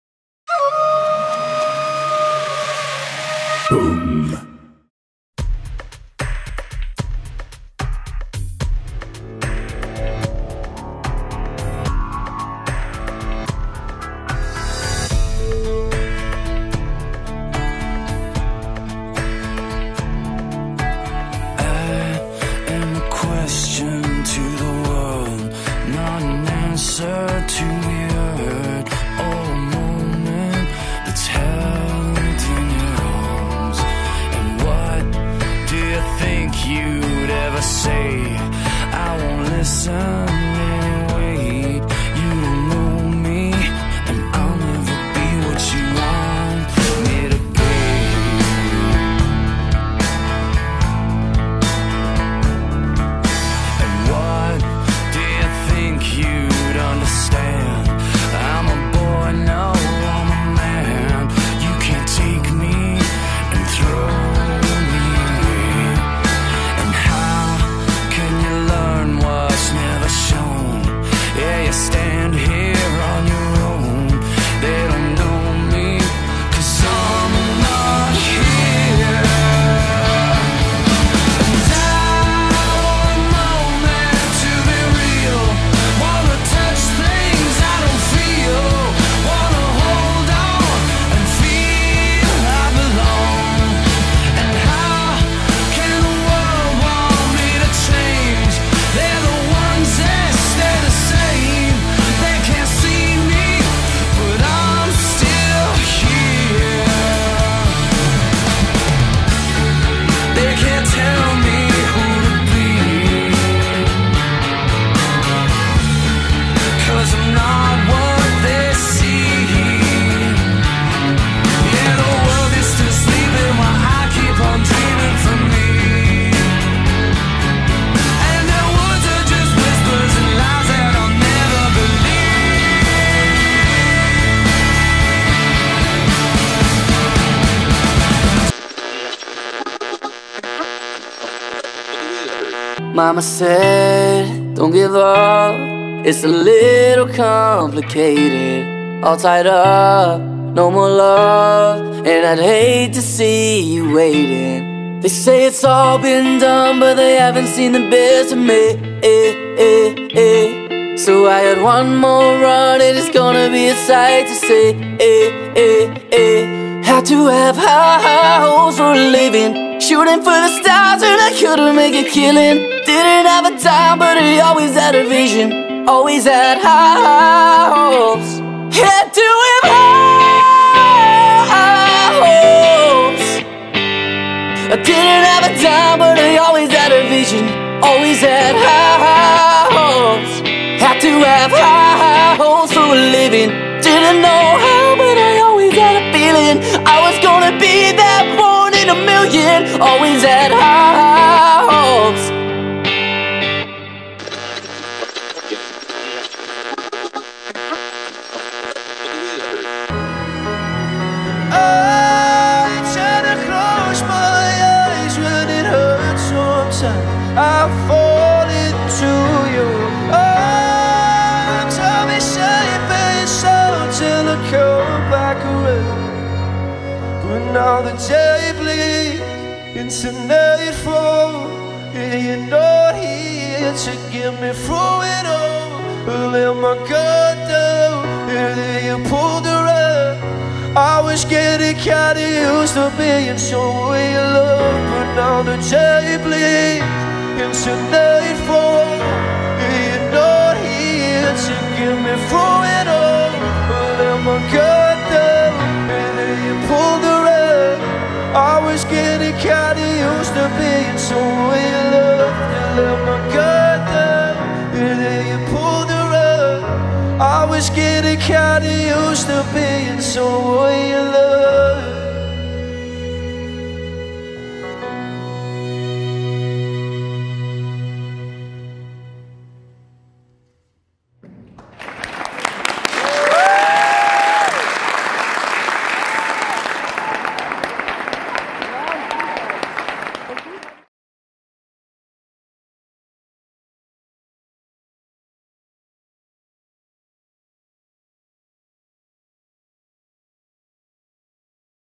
"Merci pour la danse"-Ballettgala 2019-Ballettschule Muckenthaler-Show by Robert Dream Hobelsberger
"Merci pour la danse" Ballettgala 2019, Ballettschule Muckenthaler Contemporary Show choreographed & danced by Robert "Dream" Hobelsberger

for having me !!!